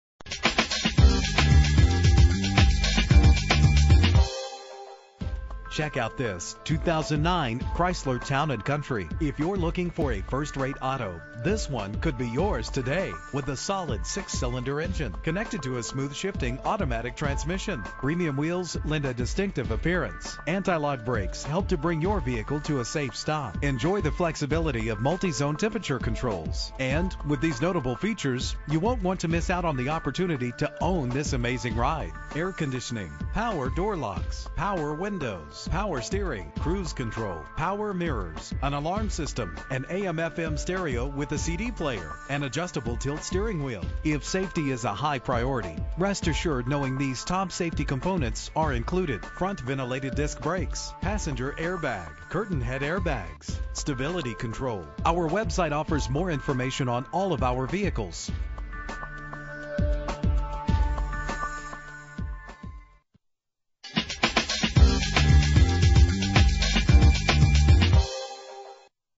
2009 Chrysler Town & Country - Cicero NY
Year: 2009
Make: Chrysler
Model: Town & Country
Engine: 3.8 liter 6 cylinder 12 valve
Transmission: Automatic
Exterior: Black
Miles: 23175
Driver's Village